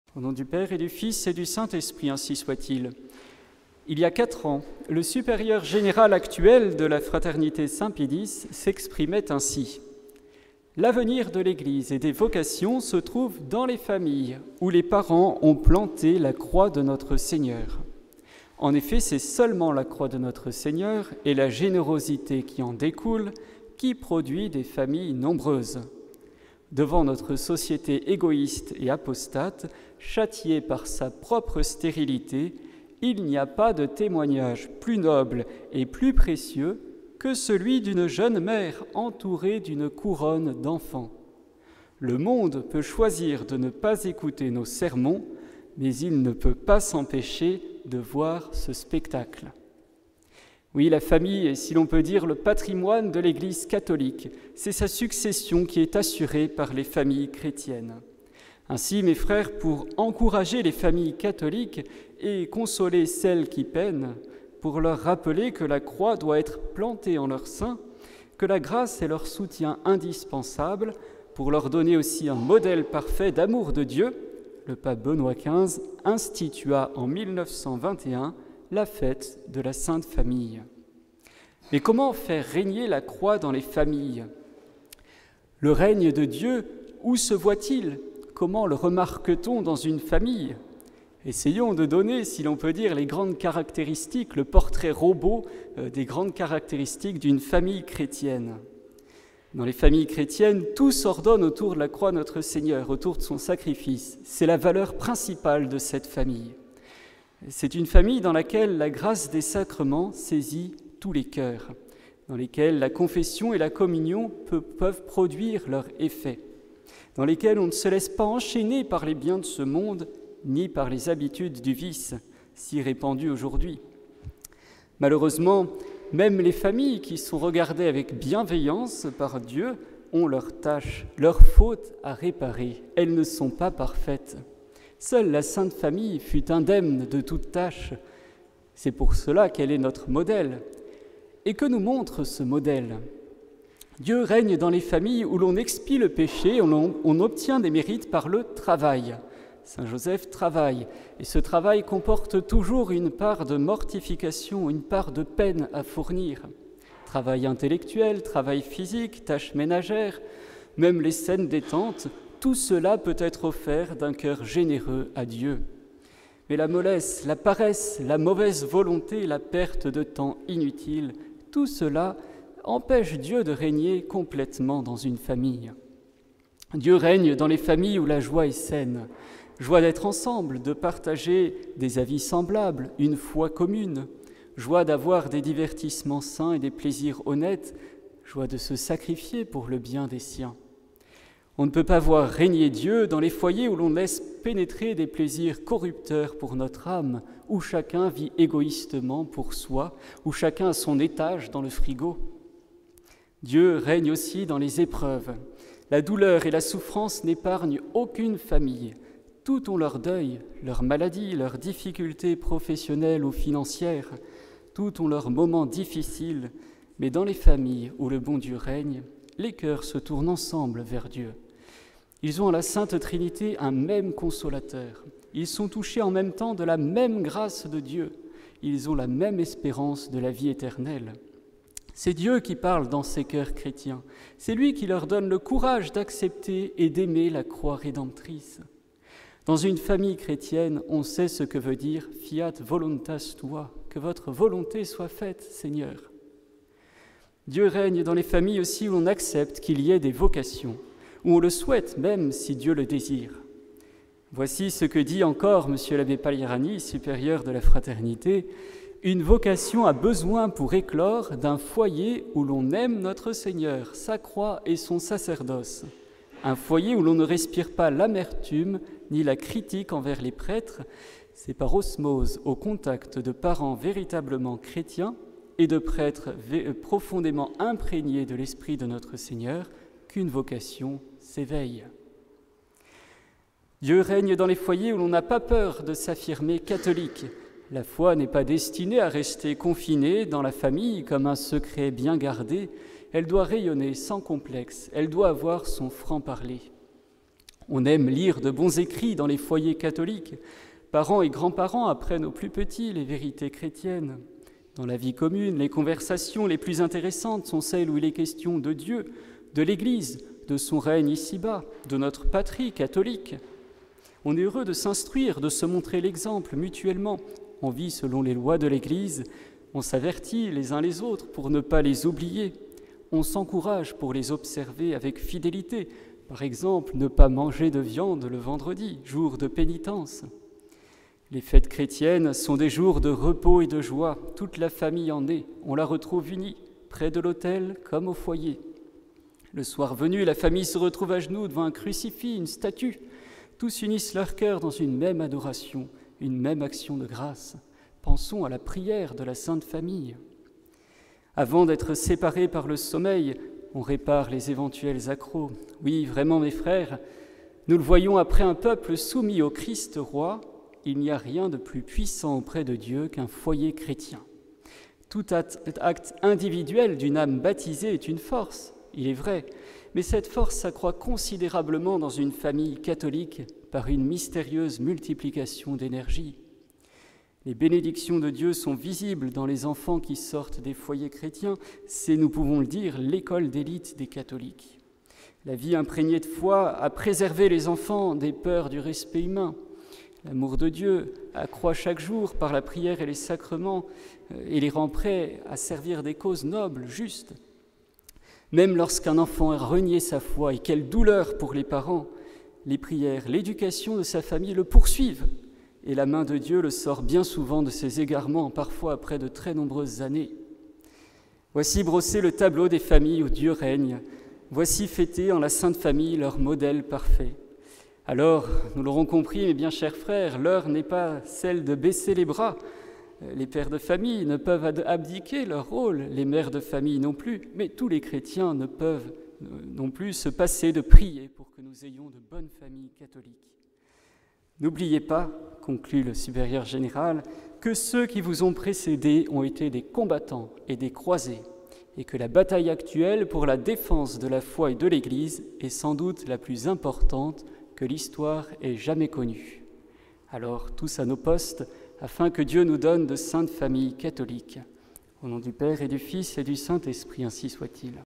Faire régner la Croix dans nos familles - Abbé Hachette
Sermon du 9 janvier 2022 donné à l’occasion de la fête de la Sainte Famille par M. l'abbé Gabin Hachette (FSSPX).